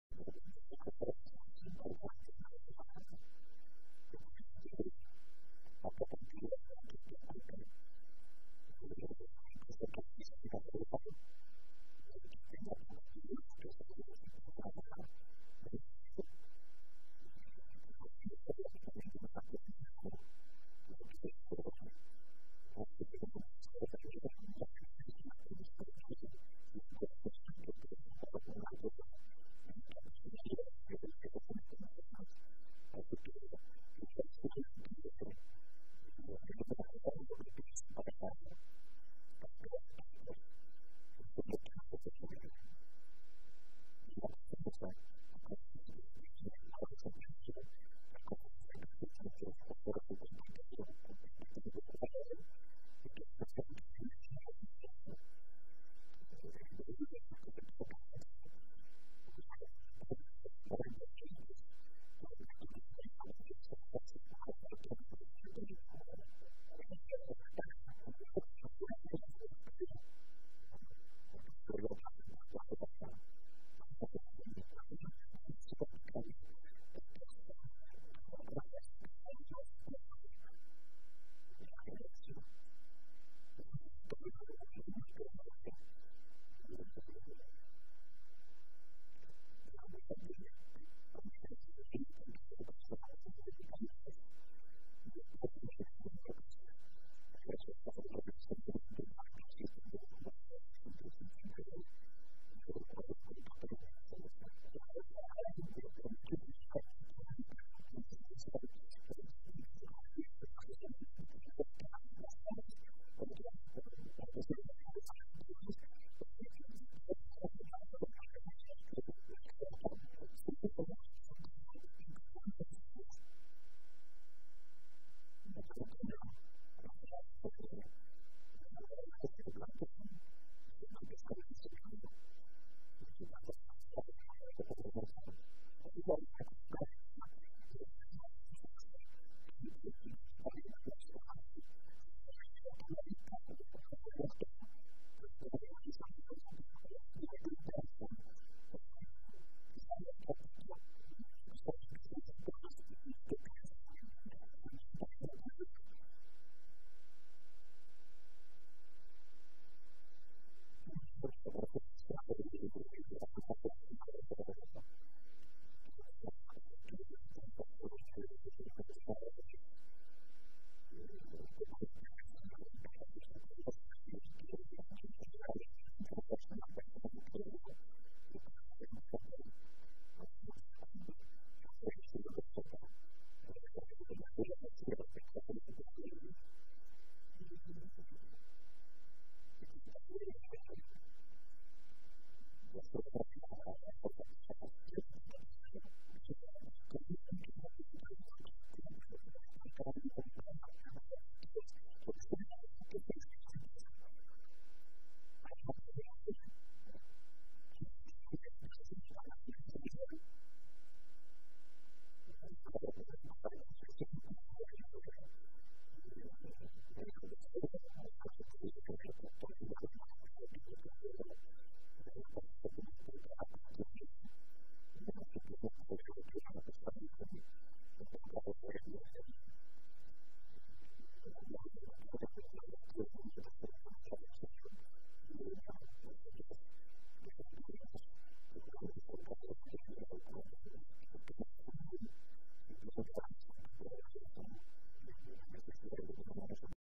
Alejandro Sabella, sobre la lista del Mundial: "La idea es respetar el grupo" (HD)
El entrenador del seleccionado, en diálogo con Sportia, dio a entender una vez más que es muy difícil la aparición de un nombre nuevo. "Tratar de hacer honor a la palabra", aseguró.